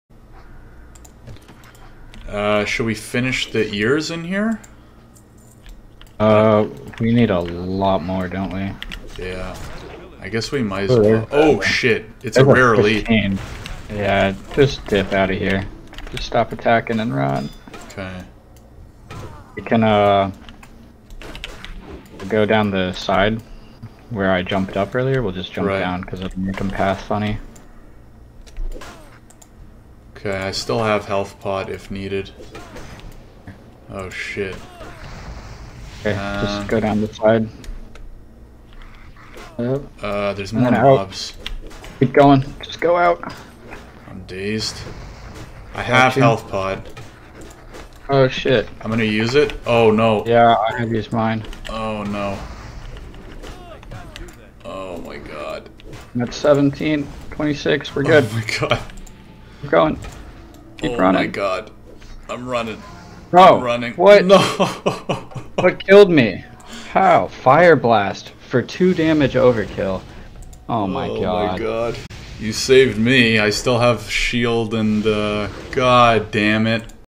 Duo death - more caves, more rare elites
A rough overpull as a duo, and a big rip.

Please make sure to like/follow for more hardcore content!